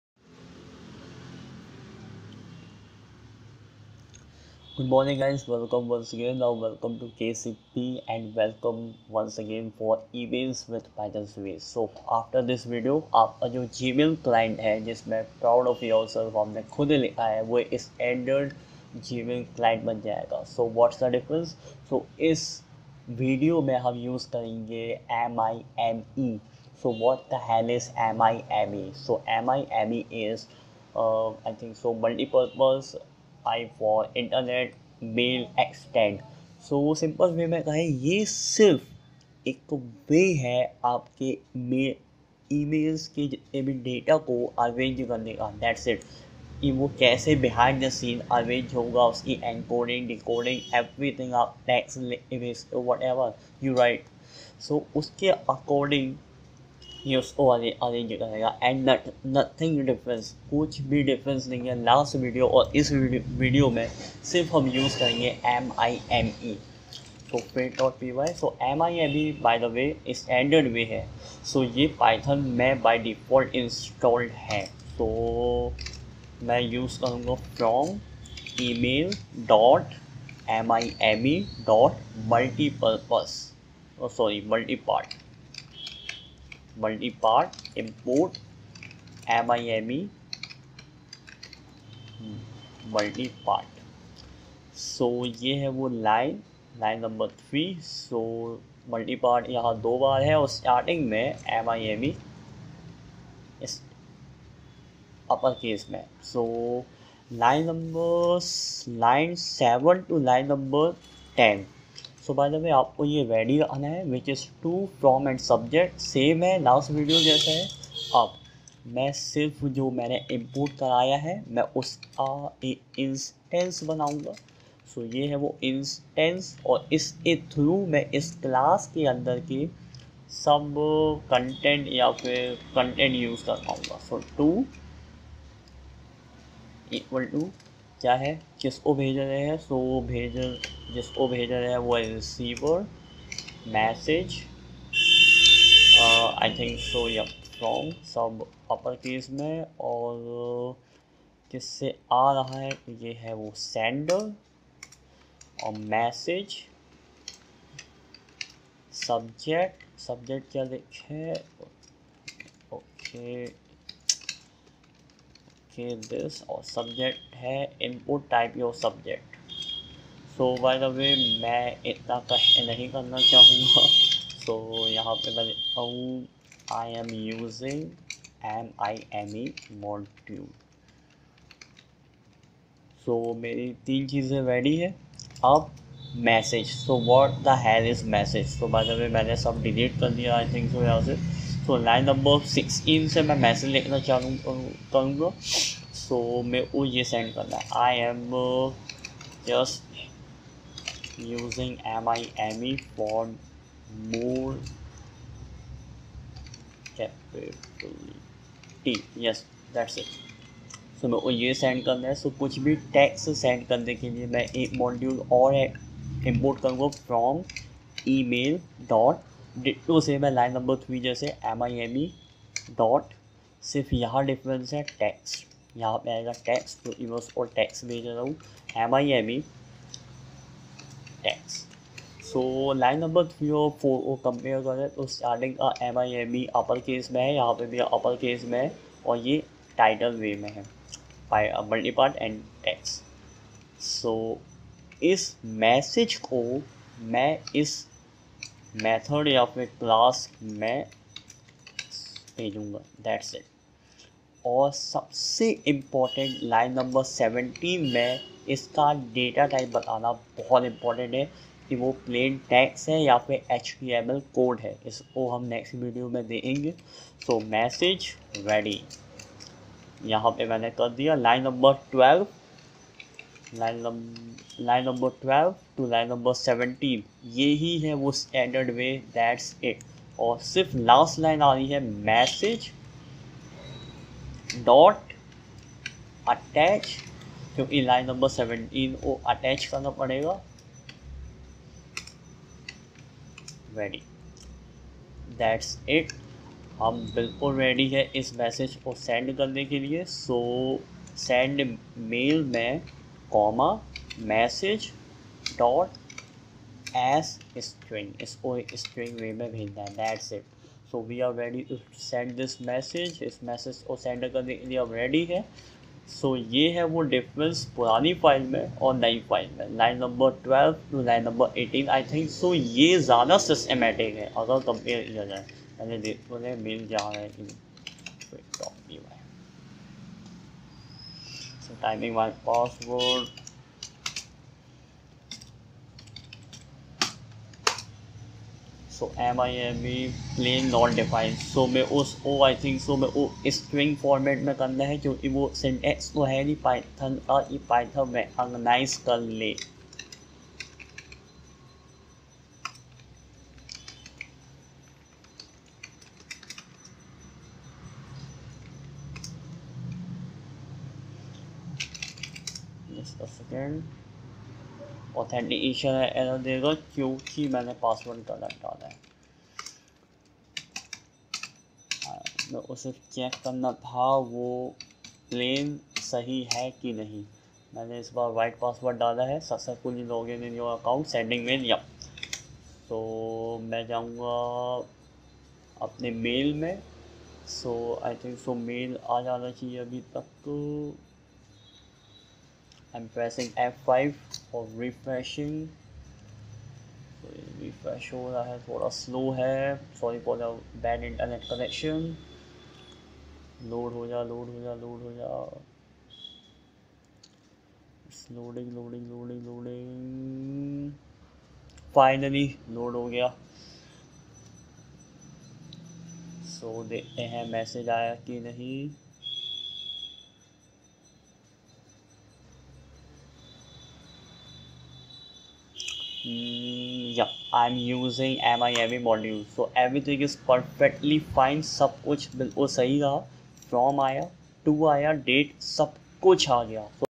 Emails With PYthon - 5 MIME Standard Email Format
In this video you will get not only information about MIME module for Standard Email arrangement but also how to implement it in our python code for sending emails